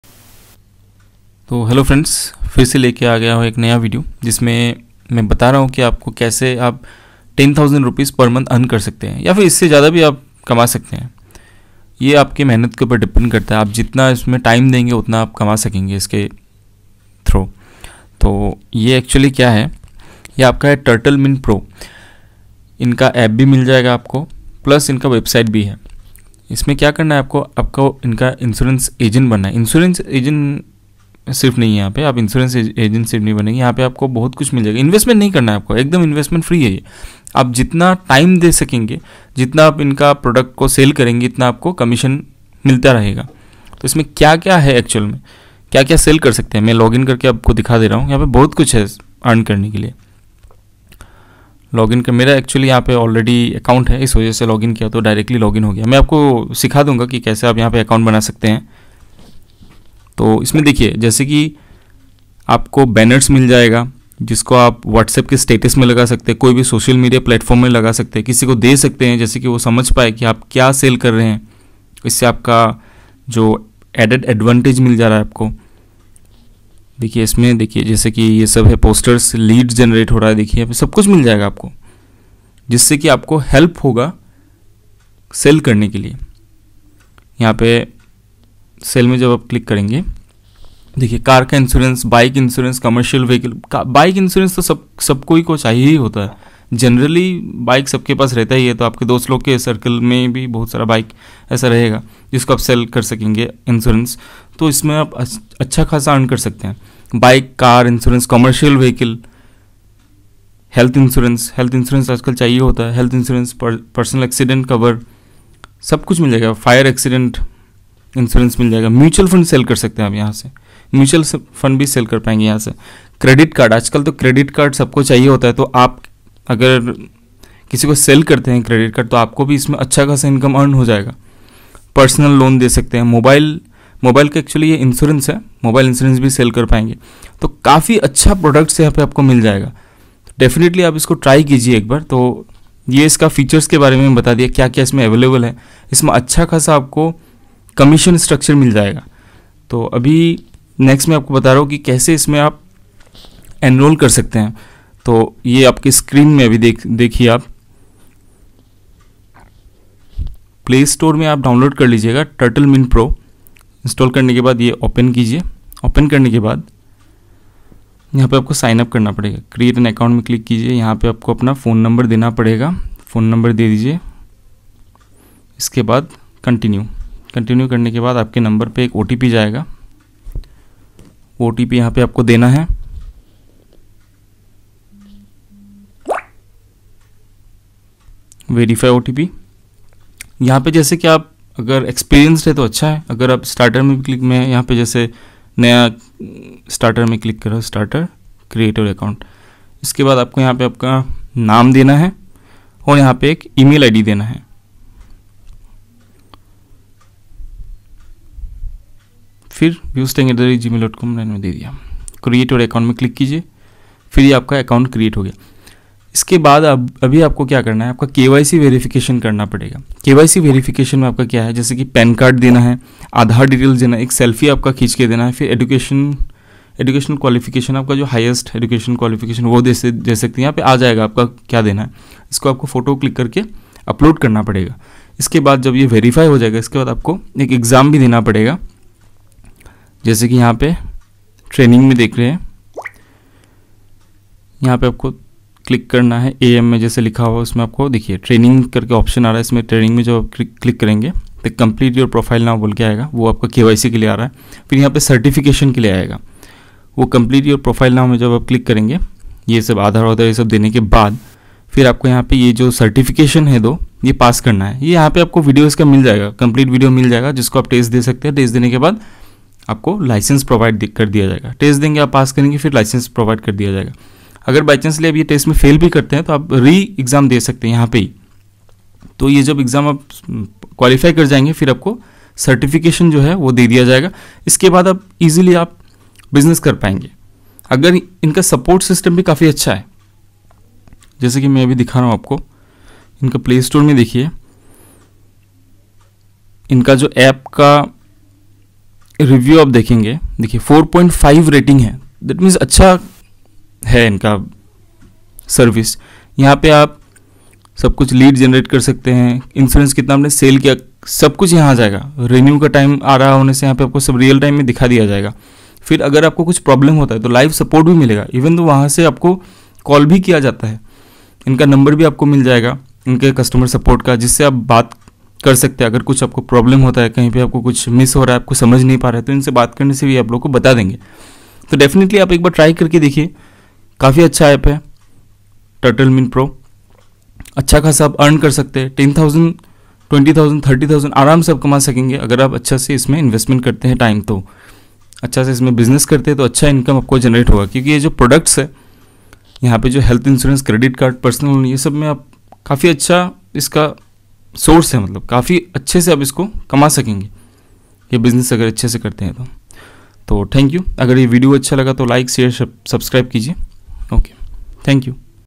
Bina Investment kare Rs 10000/- kamae har mahine.
Bina investment kare Rs 10000/- kamaeye. Jitna jeyada aap kam karenge utna aaap jeyada kama paenge Turtlemint Pro aap se.